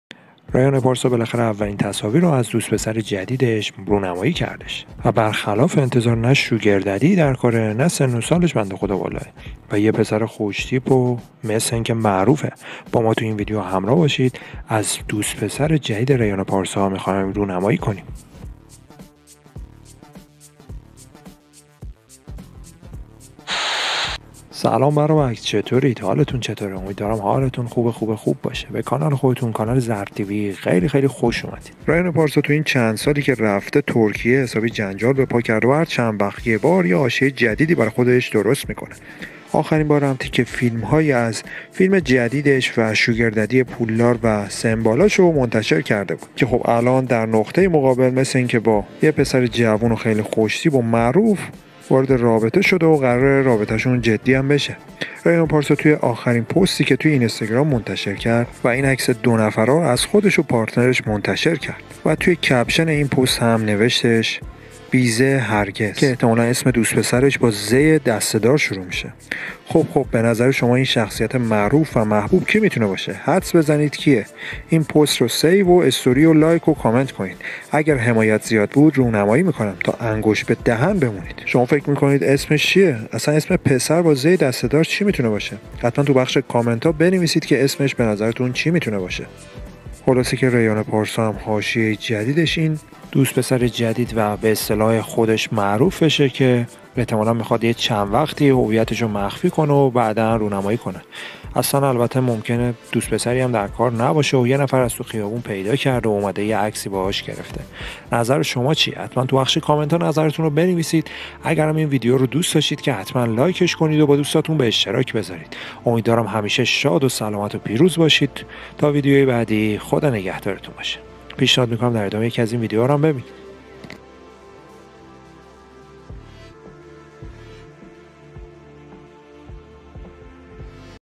عکس های جدید لو رفته دو نفره از ریحانه پارسا و دوست پسر جوانش | عکس های جنجالی ریحانه و دوست پسرش
سلام دوستان
عکس های جنجالی و جدید از ریحانه پارسا و دوست پسرش که لو رفته

ماجرای دوست پسر و پارتنر جدید و جنجالی و معروف ریحانه پارسا
توی این ویدیو ماجرا شو بهتون میگم

دوست پسر جدید و معروف ریحانه پارسا | تصاویر دو نفره از ریحانه و پارتنرش